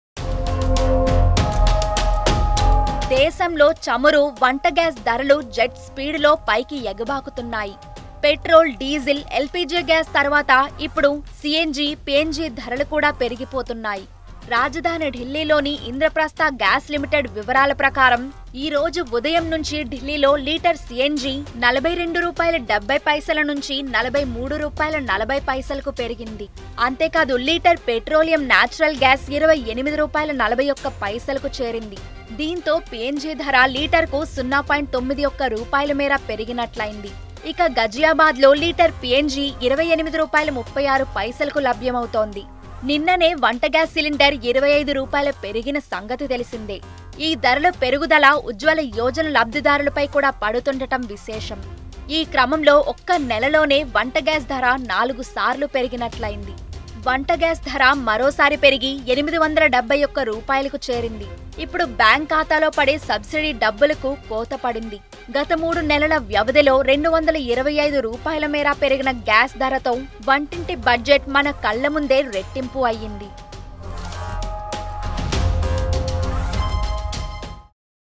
Price of CNG, PNG hiked from today, Petrol and Diesel prices remain stable - TV9
Price of CNG, PNG hiked from today, Petrol and Diesel prices remain stable

Credit